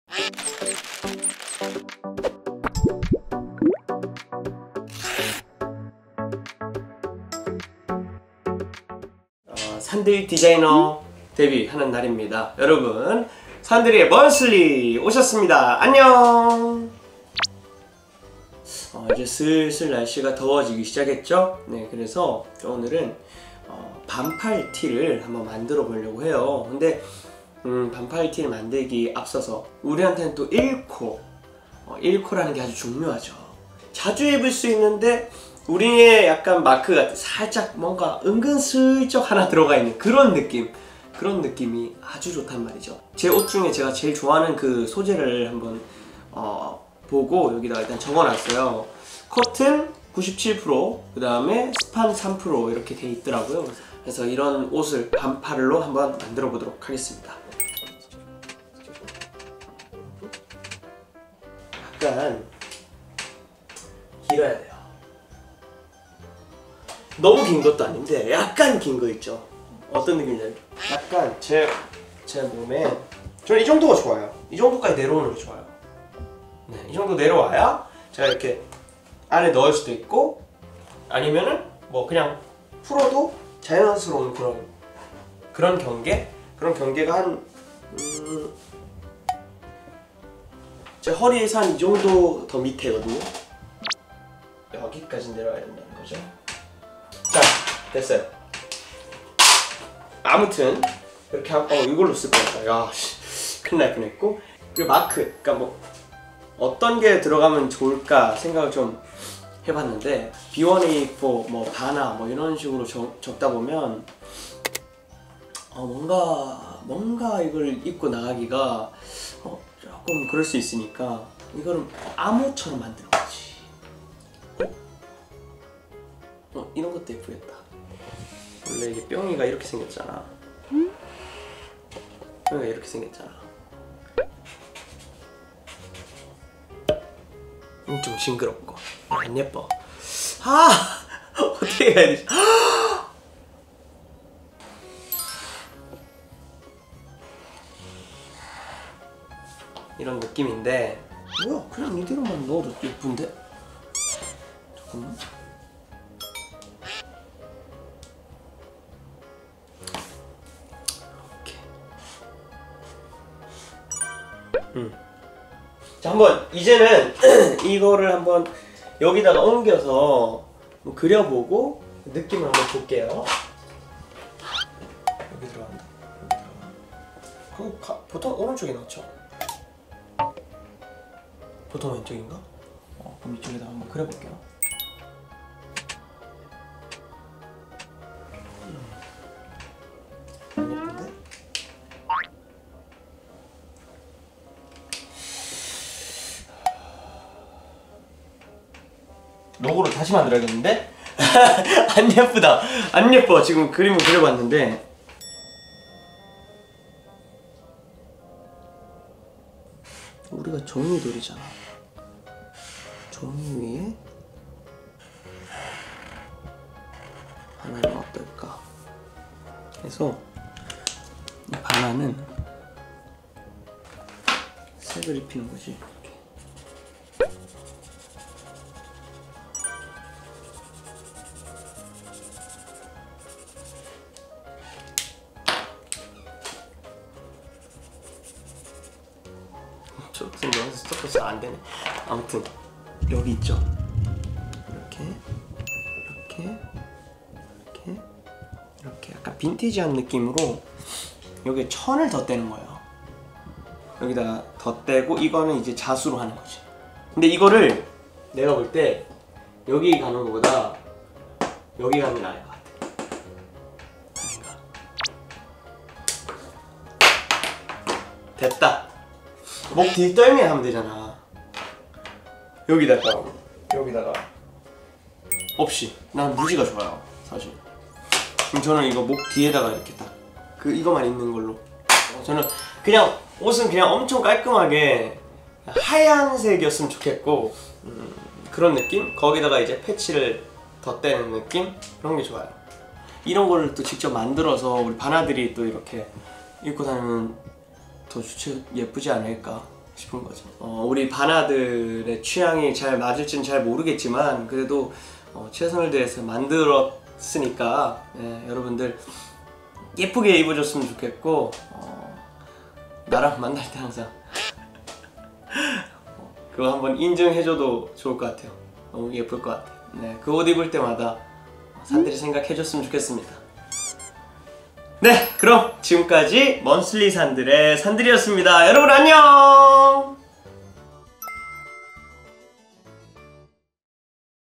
[MONTHLY SANDEUL] #6 산들 디자이너의 굿즈티 만들기!
산들 디자이너의 작품이 궁금하다면?

(* [MONTHLY SANDEUL]의 모든 영상은 산들의 군입대 전에 촬영된 영상입니다.)

B1A4 Official Channel